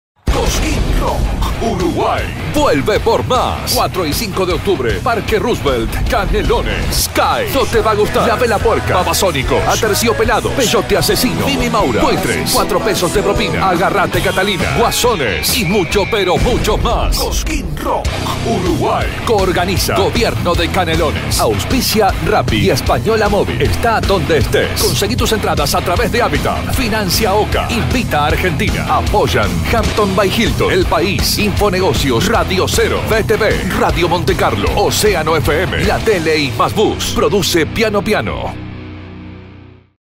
Cosquín Rock Uruguay 2019 se realizará los días 4 y 5 de octubre
El principal festival de rock de Argentina llega a Uruguay con su segunda edición, los días viernes 4 y sábado 5 de octubre en Canelones.